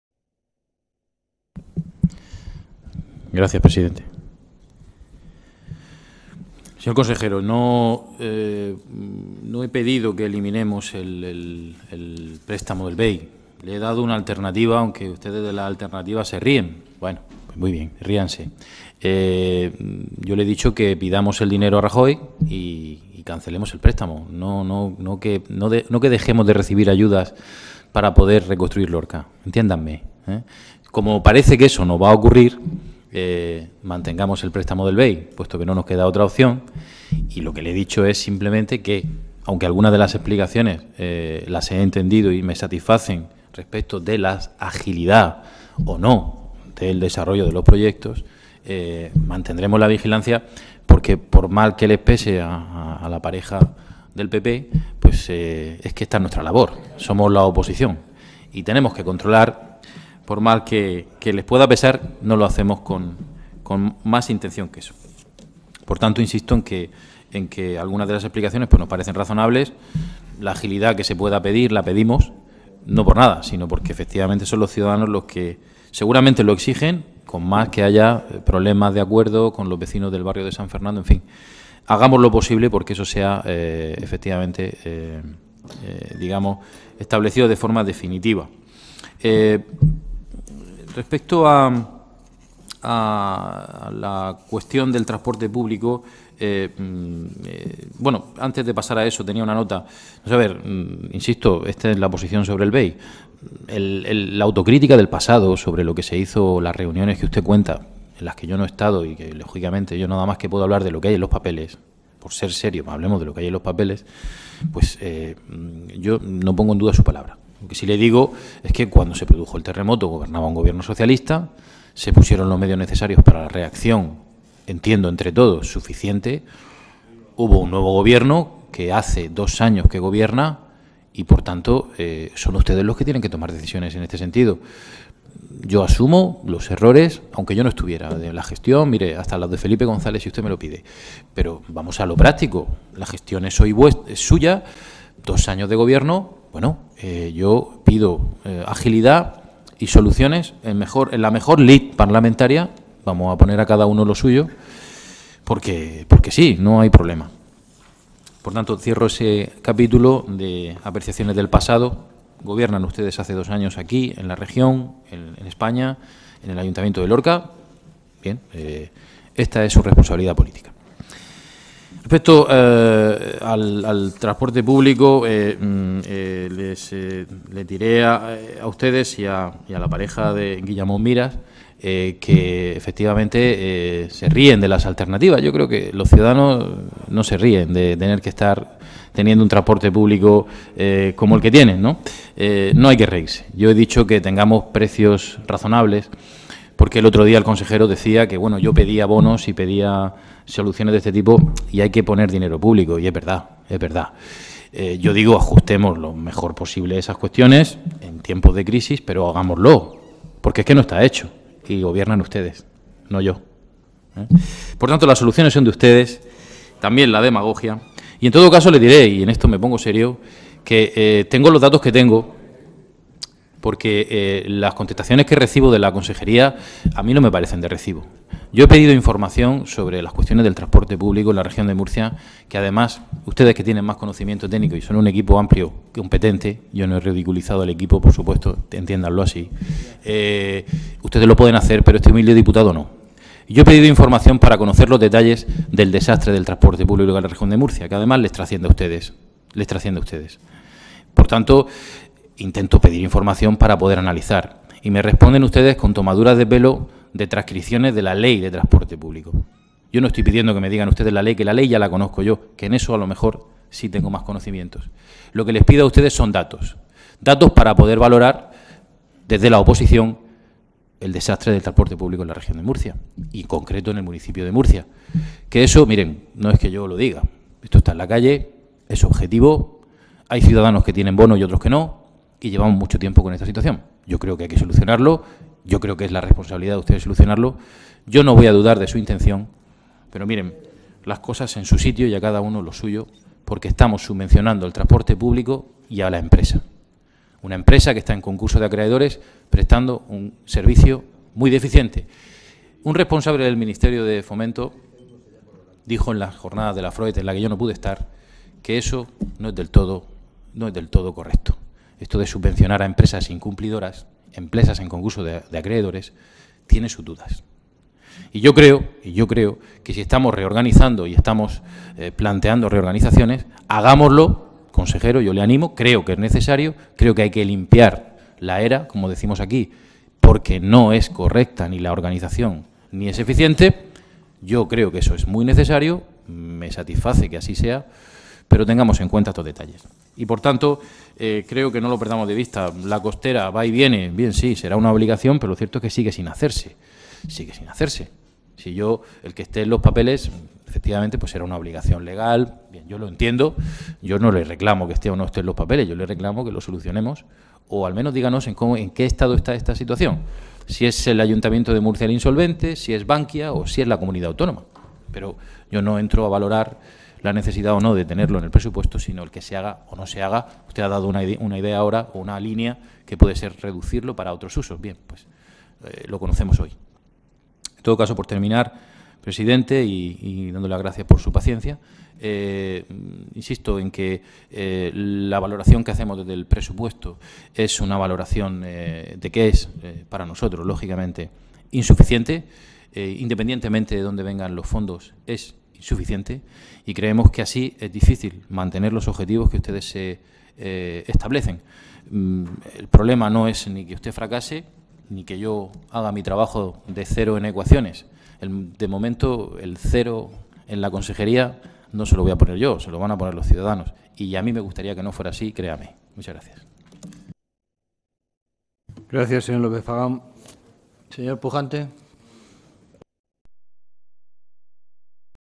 López Pagán, Joaquín (G.P. Socialista). Turno final de intervenciones.
Proyecto de Ley de Presupuestos 2014. Sección 14. 6/11/2013. Consejería de Obras Públicas y Ordenación del Territorio